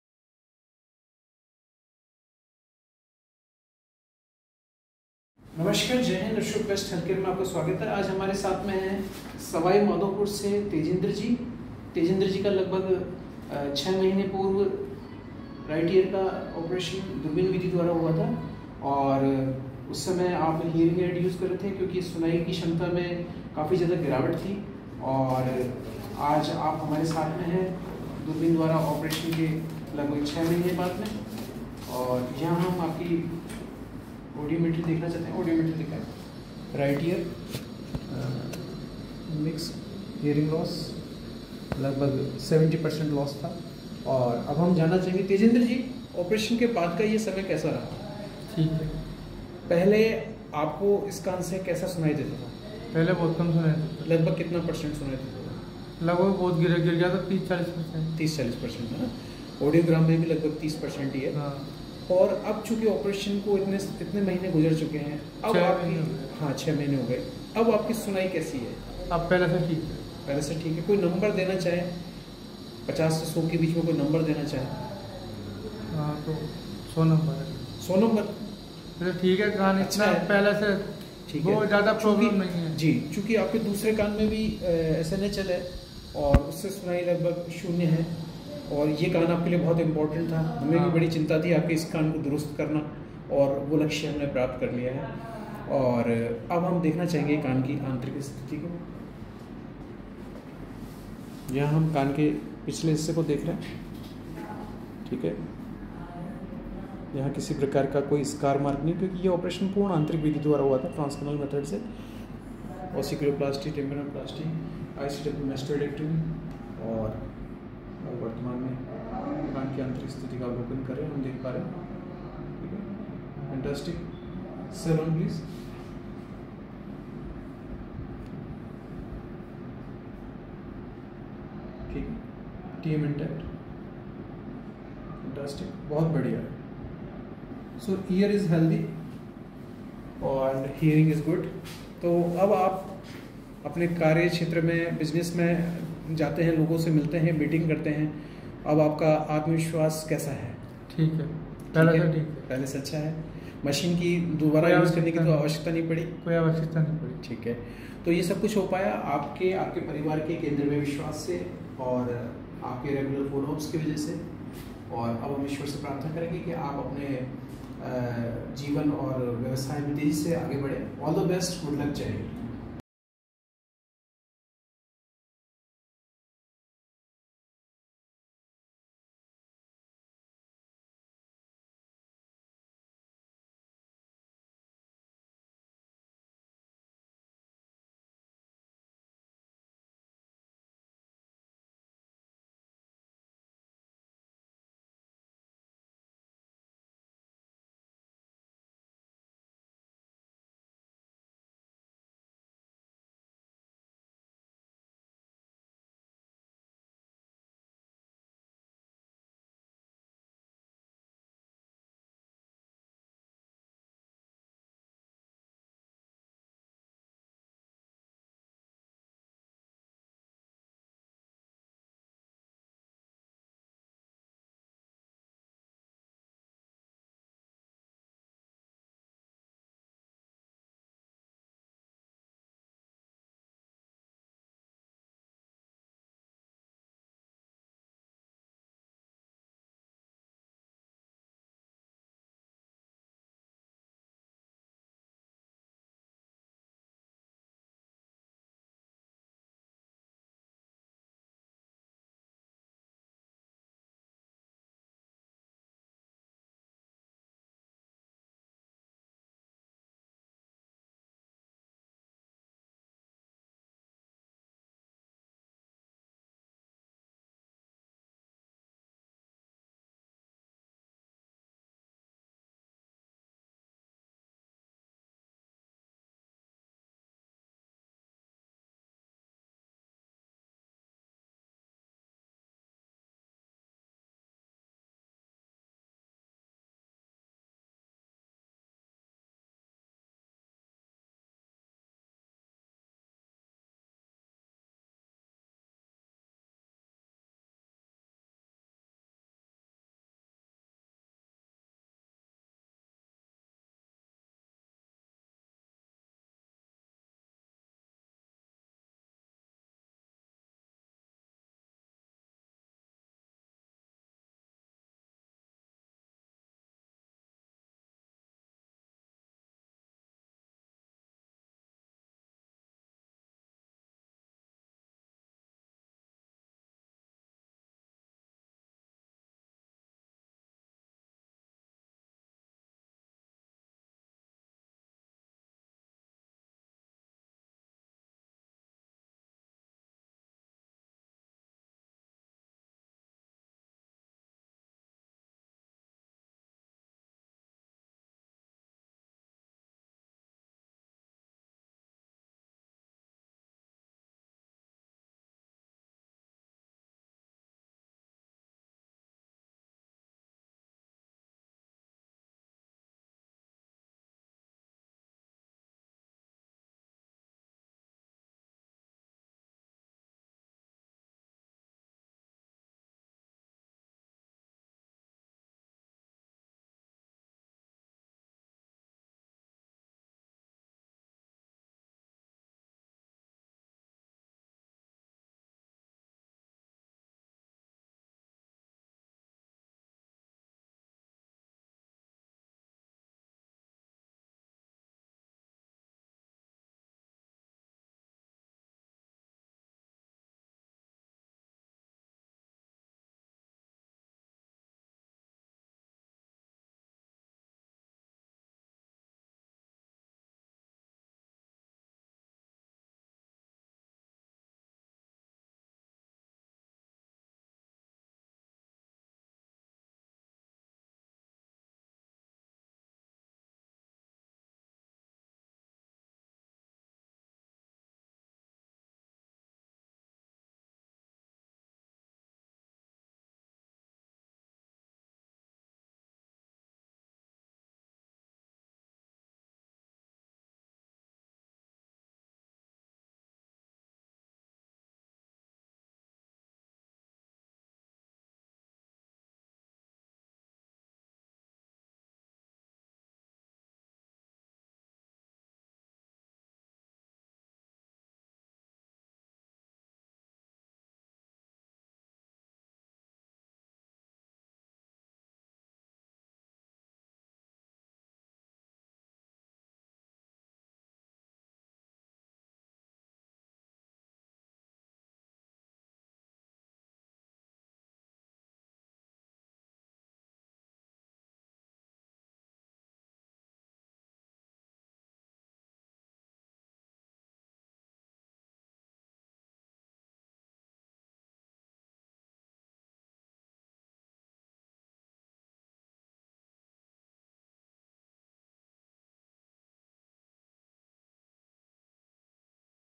सवाईमाधोपुर राज के युवा को मिला ऑपरेशन के बाद सुनाई की मशीन से छुटकारा ? good hearing after surgery
अशोक BEST हेल्थकेअर जयपुर,ज्योति नगर गेट नंबर 1, राजस्थान विधान सभा के पास